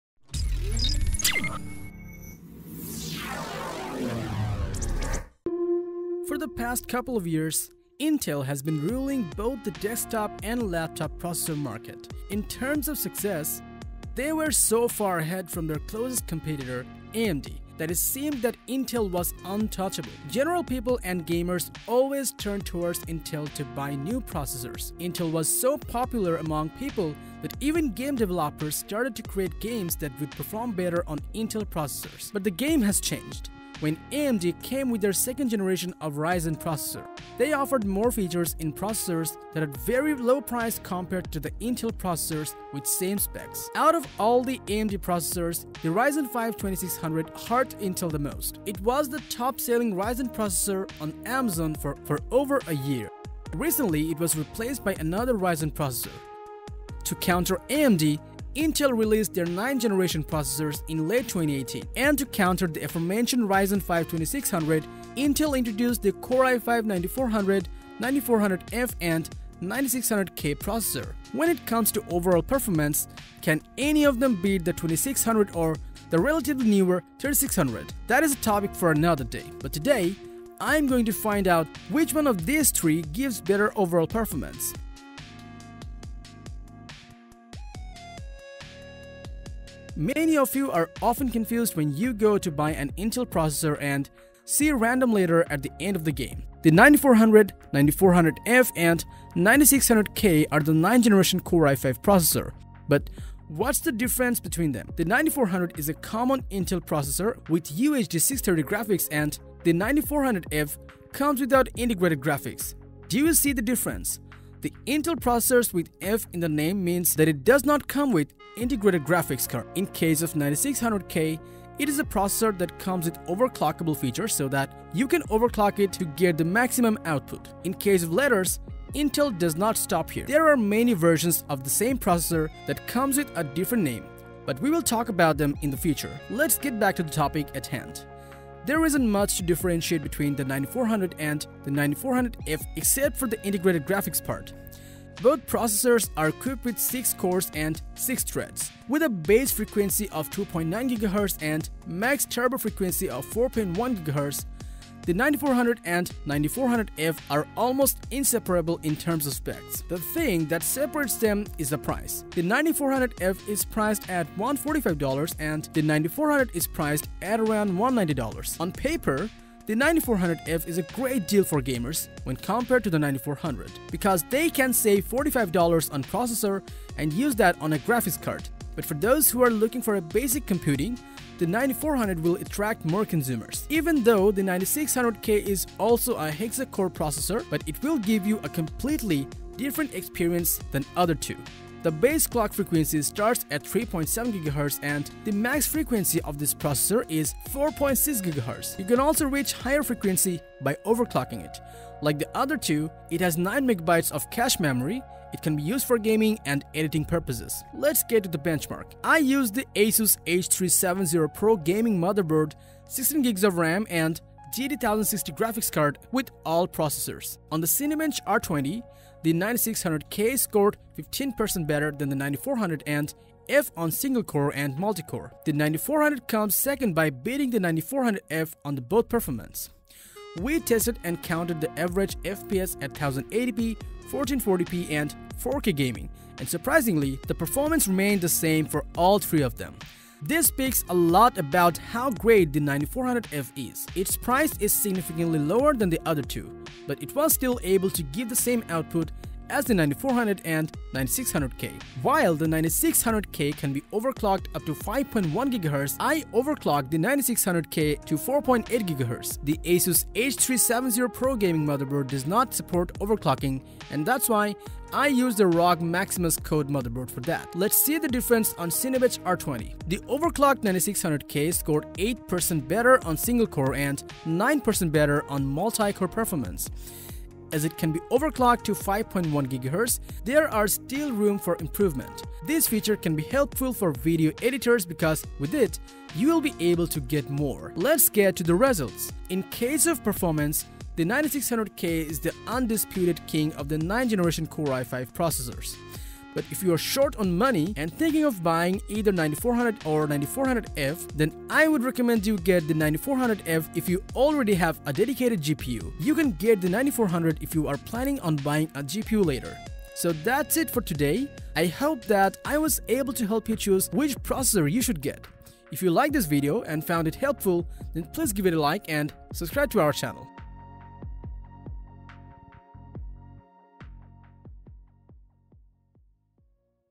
Intel i5-9400 vs i5-9400F vs i5-9600K Comparison | Benchmarks | Test Review
For the past couple of years, Intel has been ruling both the desktop and laptop processor market. In terms of success, they were so far ahead from their closest competitor, AMD that it seemed that Intel was untouchable. To counter AMD, Intel released the 9th gen processors in late 2018. And to counter the aforementioned ryzen 5 2600, Intel introduced the core i5 9400, 9400f and the 9600k processor. Today I am going to find out which one of these three gives better overall performance.